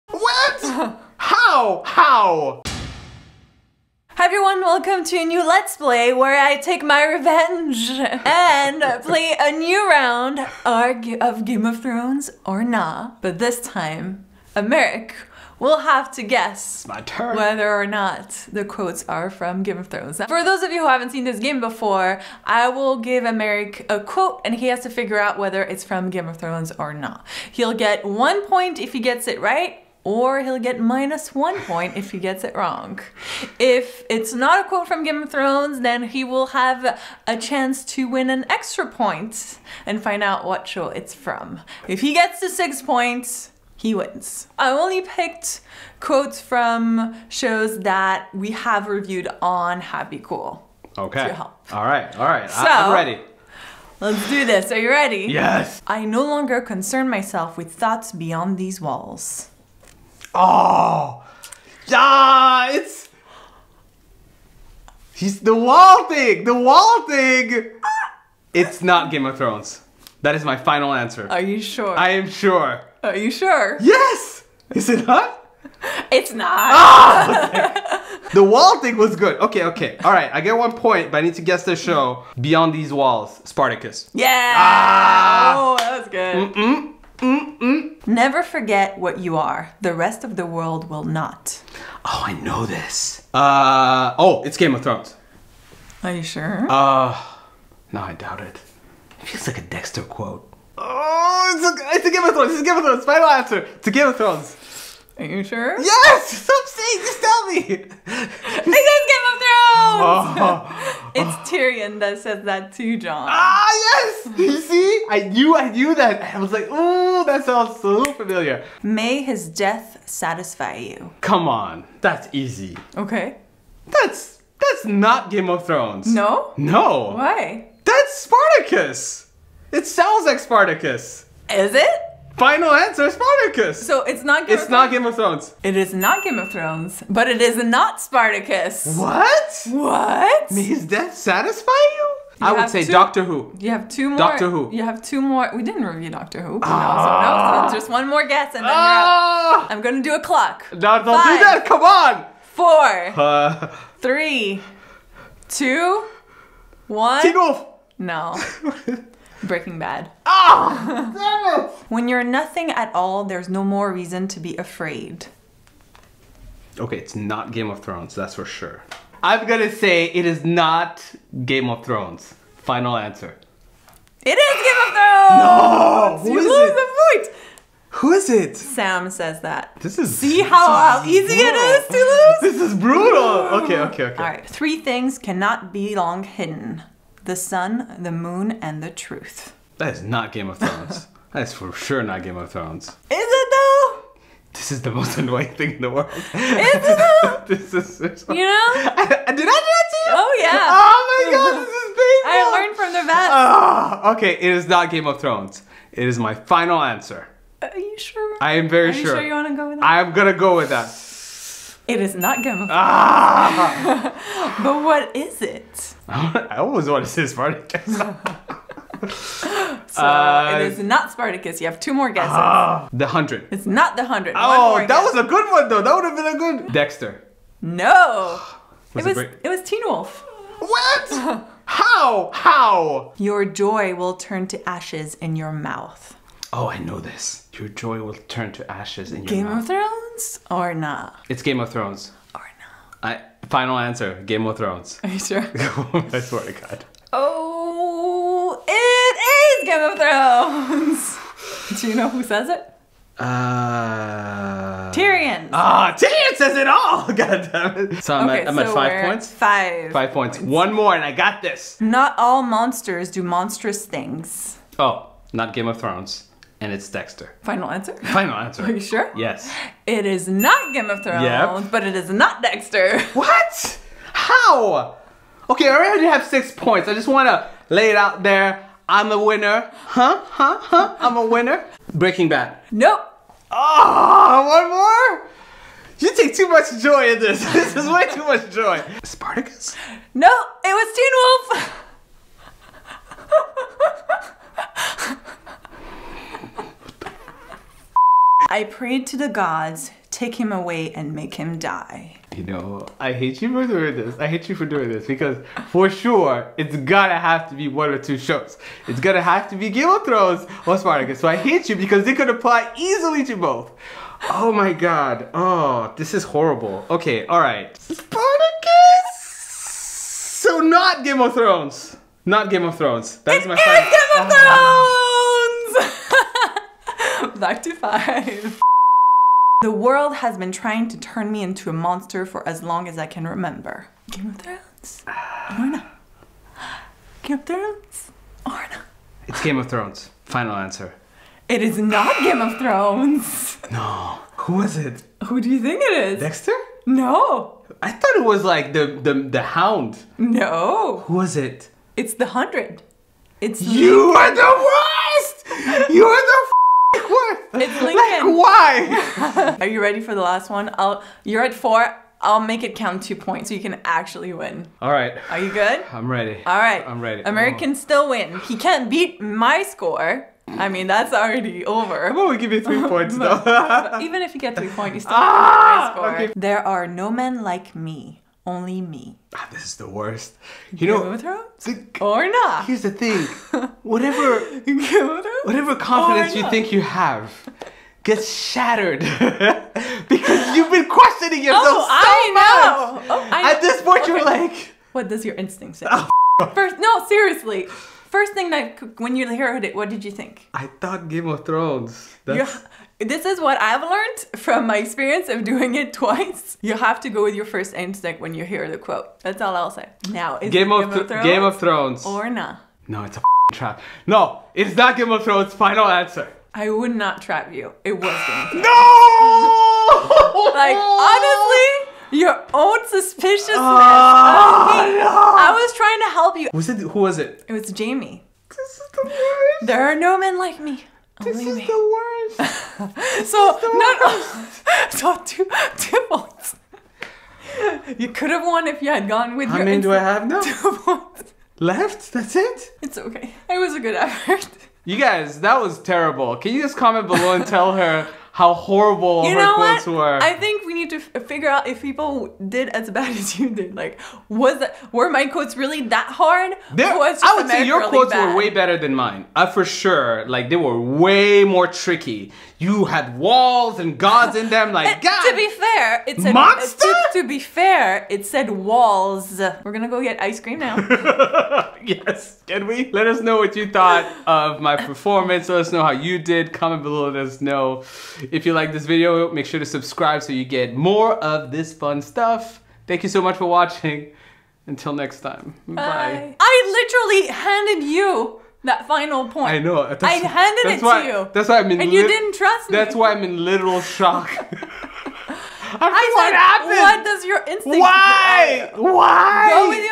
Game of Thrones or Nah? TAKE 2 (Let's Play Game)
It's time for Sandrine to take her revenge in another round of Game of Thrones or Nah. Americ will have to correctly identify whether a quote is from Game of Thrones or another TV Show. Will he make it? And more importantly will he beat Sandrine's score?

Quotes used on this video:

- "I no longer concern myself with thoughts beyond these walls." -Spartacus

- "Never Forget what you are. The rest of the world will not." -Game of Thrones

- "May his death satisfy you." -Breaking Bad

-"When you're nothing at all there's no more reason to be afraid." -Game of Thrones

-"Three things cannot be long hidden: the sun, the moon and the truth" -Teen Wolf

-"Your joy will turn to ashes in your mouth." -Game of Thrones

-"Not all monsters do monstrous things" -Teen Wolf

-"I prayed to the Gods 'Take him away, make him die.'" -Game of Thrones

-"The world has been trying to turn me into a monster for as long as I can remember." -The 100

-"There are no men like me, only me." -Game of Thrones